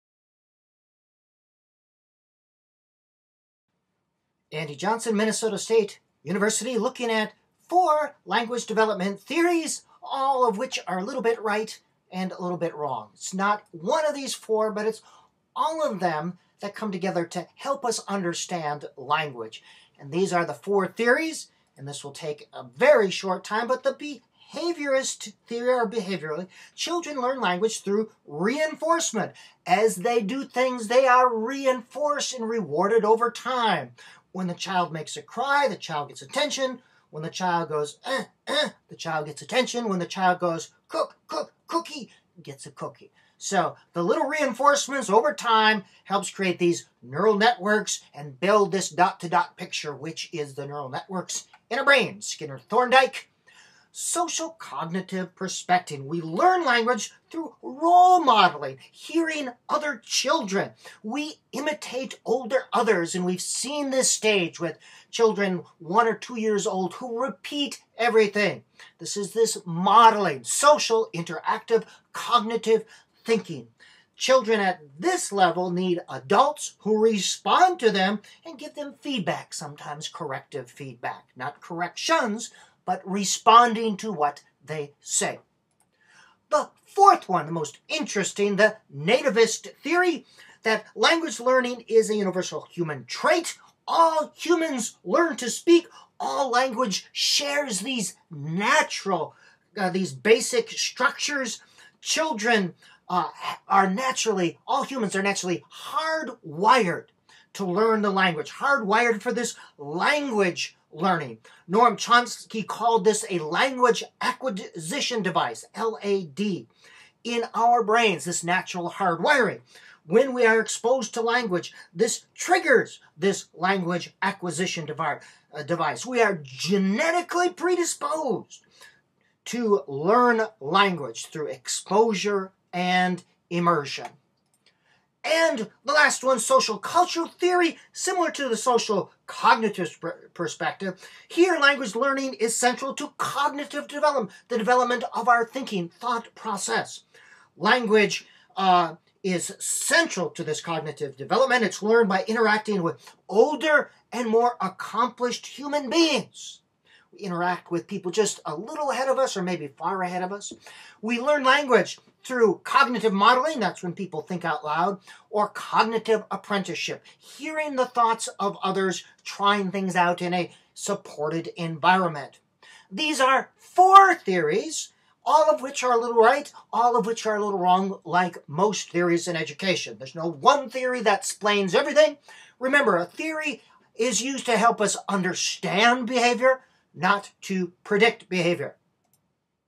language development theories.mp4
This mini-lecture describes some language development theories.  Forgive the err.  It should be Noam Chomsky not Norm Chomsky.  Dr. Andy Johnson, Reading Specialist,